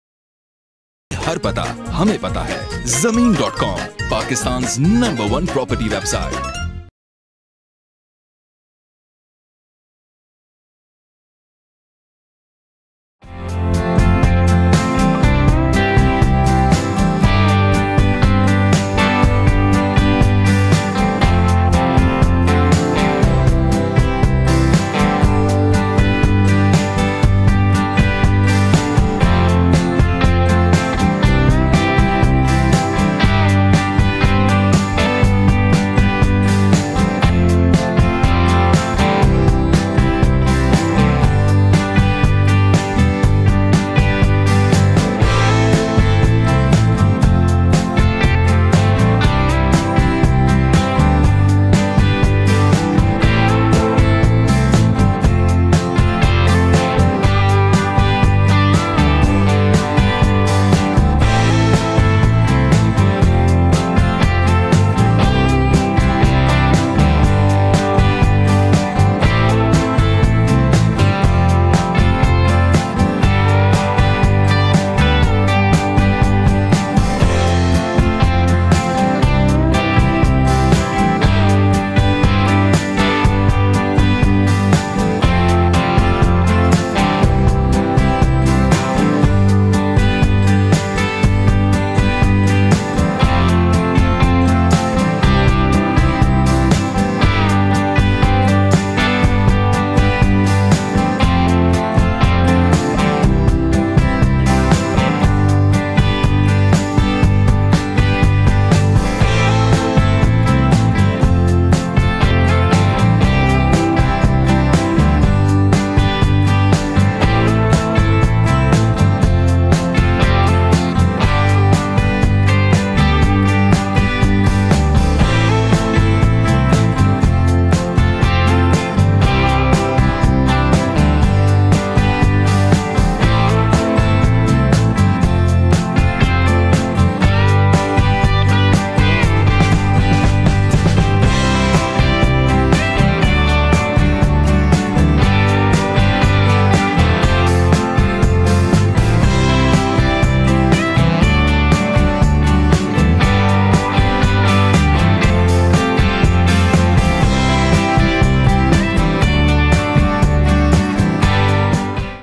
5 MARLA BRAND NEW HOUSE IS AVAILABLE FOR SALE IN BLOCK A PHASE 1 STATE LIFE HOUSING SOCIETY LAHORE
This House contains 3 bed rooms, 4 bathrooms with shower cabins
Drawing room and dining room
T.V lounge
2 Proper Kitchen
1 Store room
Powder room
Car parking
Beautiful terrace with BBQ area
1 Laundry
Near to park, Mosque and commercial market
Well-Constructed.